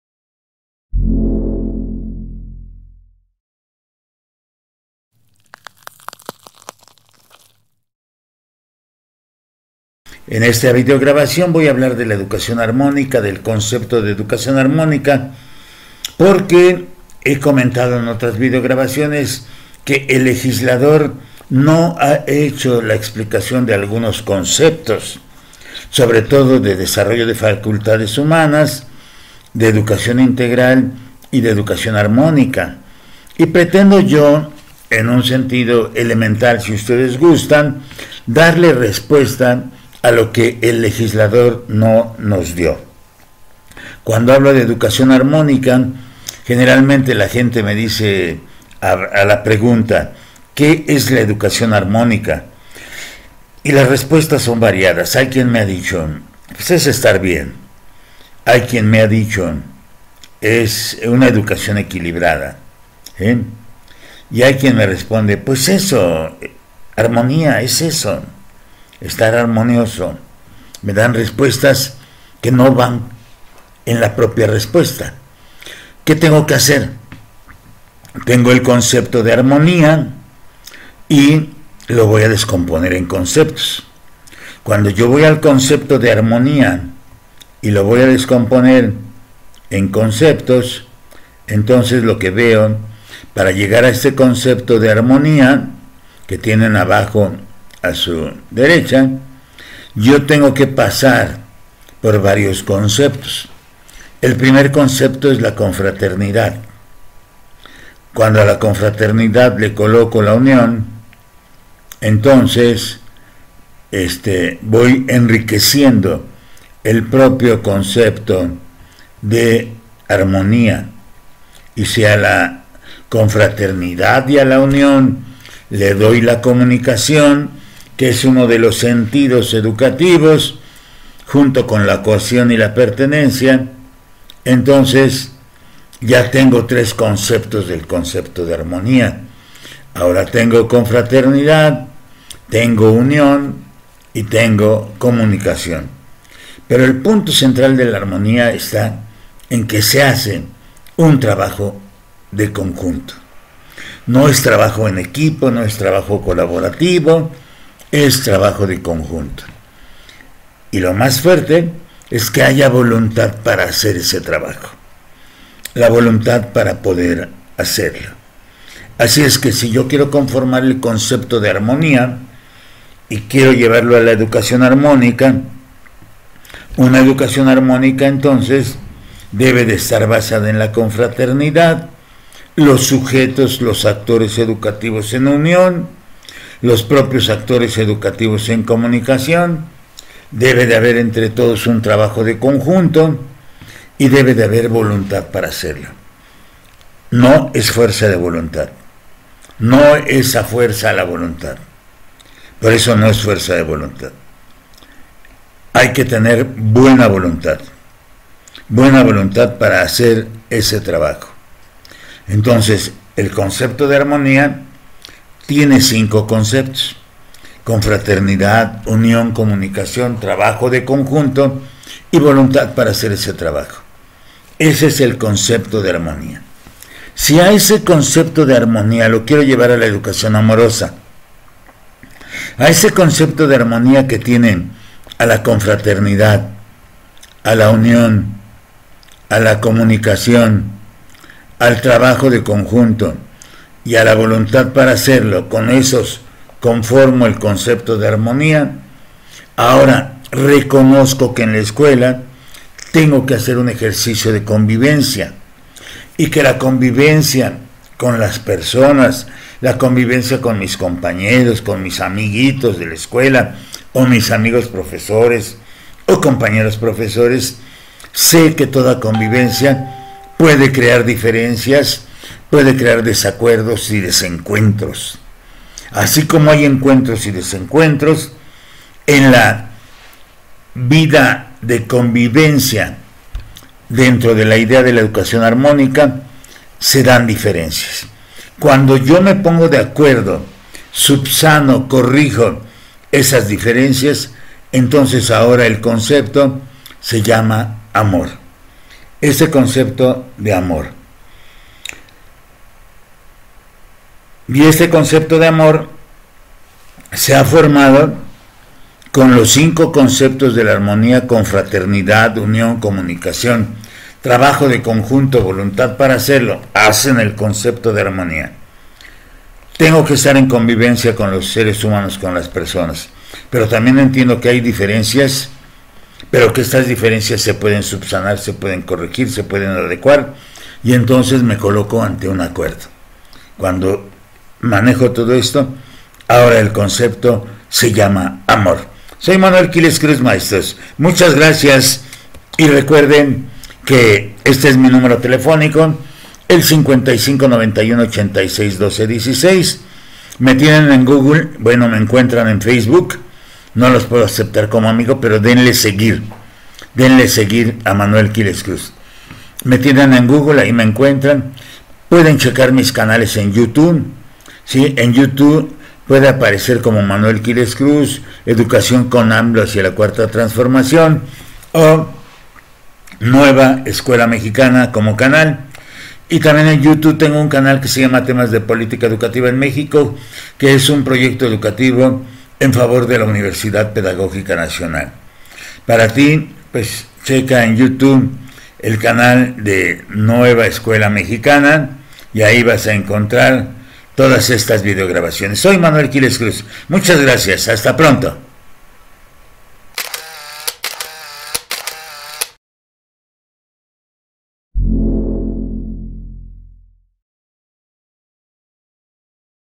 NUEVA ESCUELA MEXICANA QUILES 13 CONCEPTO DE ARMONÍA
Aquí explico el concepto de Armonía pretendiendo ayudar al legislador que no explica algunos conceptos